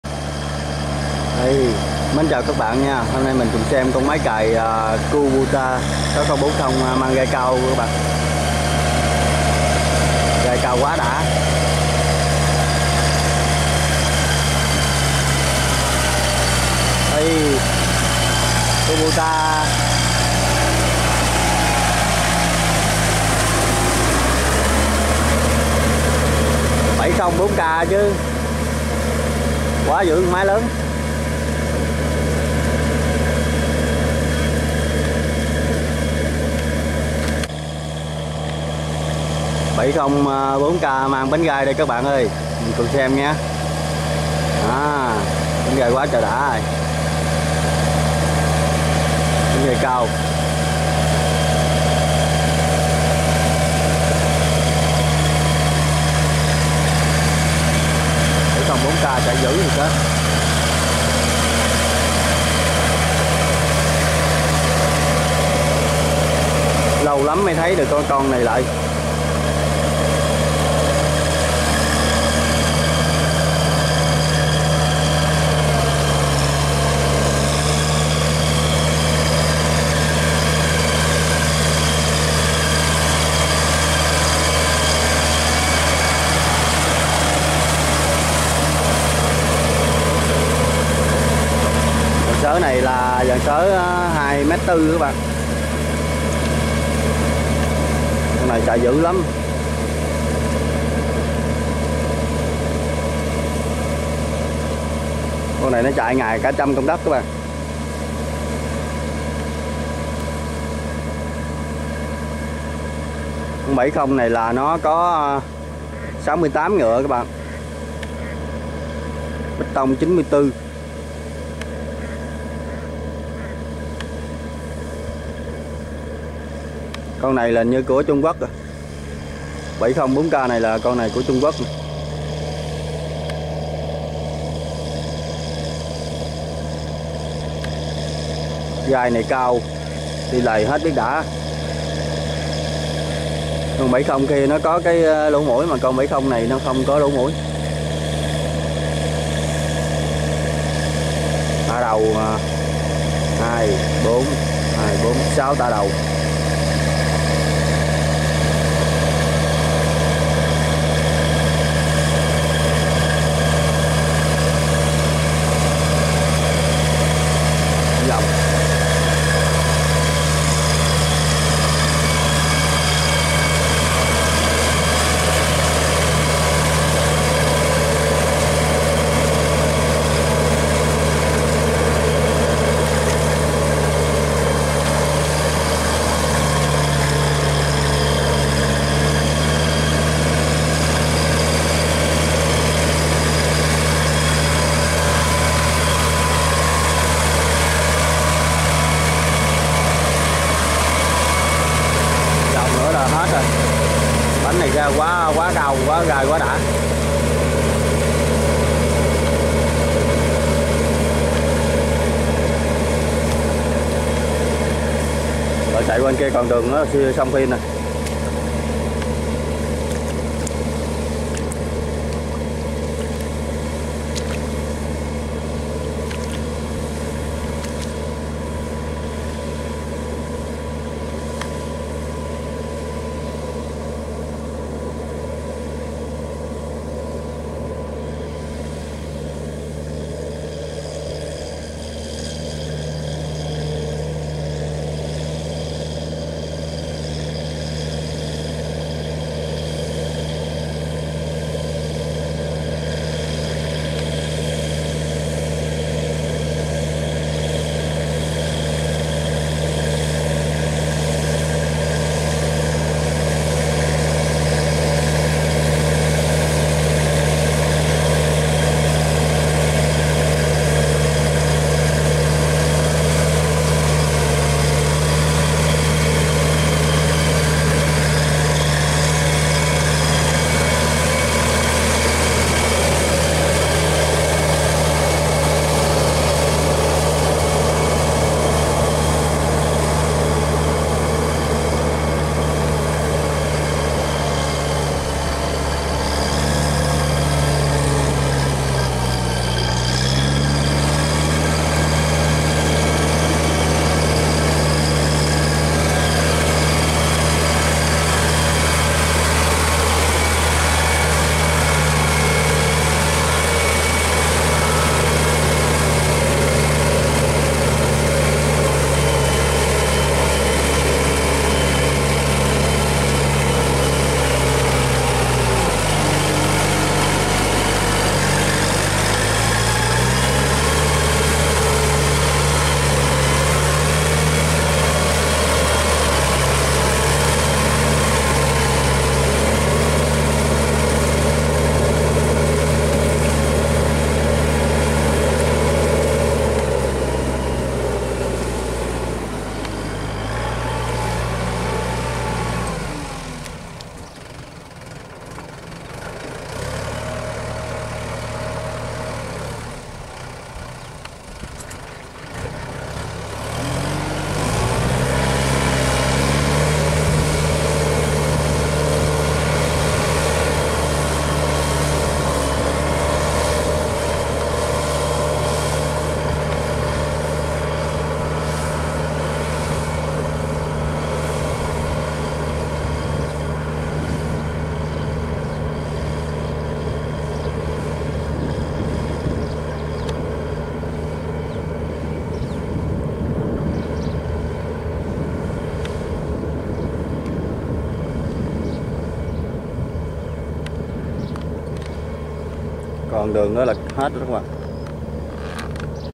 M704k Mang Bánh Gai Cao Bao Phê Với Cái Nhìn Đầu Tiên
Máy cày KuBoTa m704k chạy xới đất đồng Ba Xoài Tịnh Biên An Giang